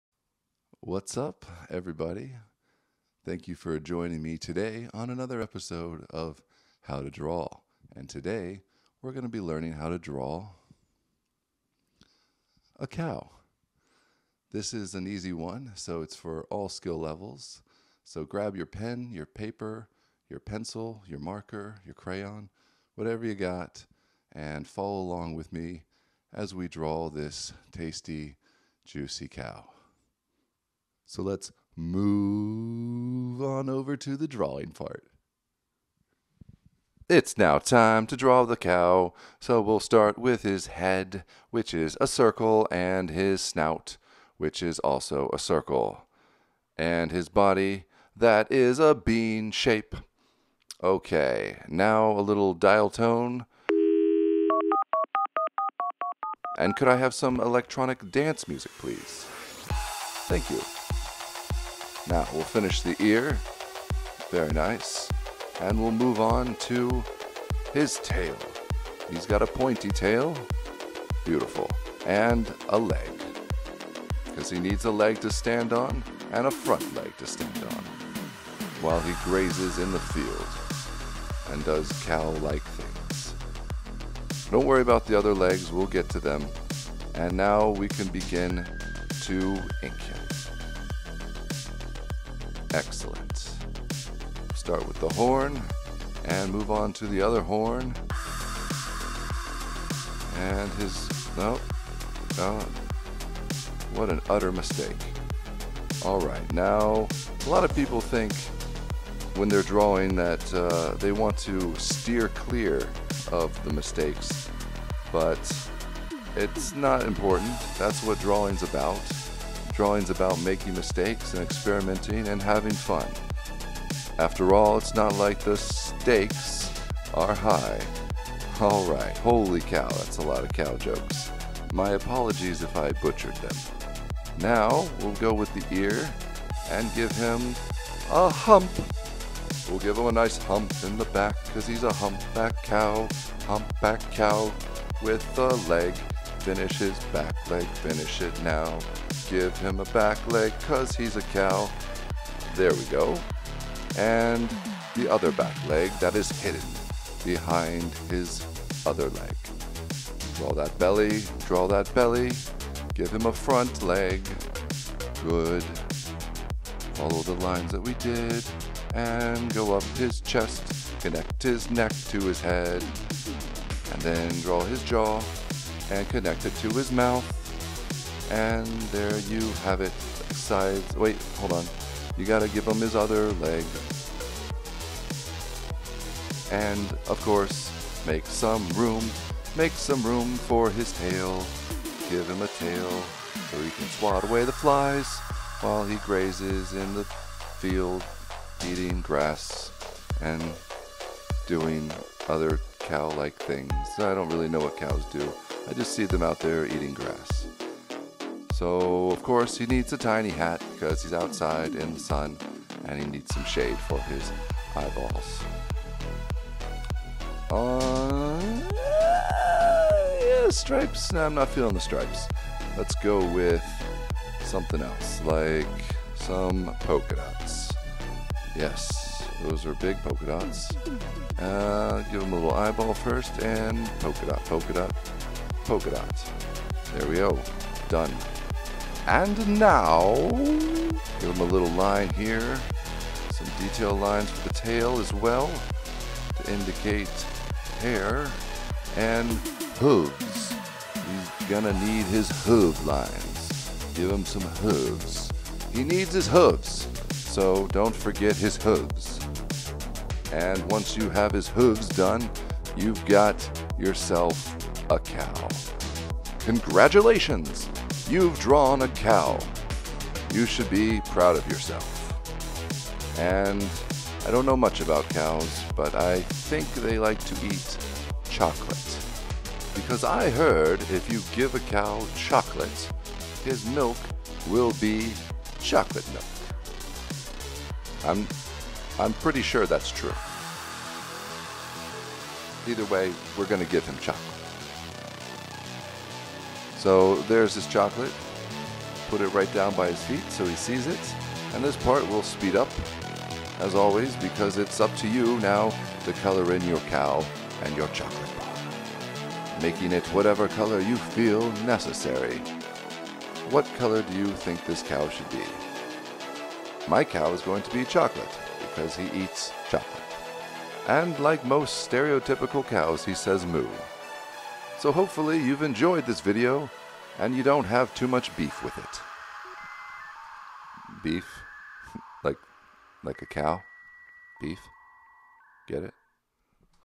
How to draw a Cow
In this video I teach you how to draw a Cow. Hopefully you can learn something from this video and practice your drawing skills.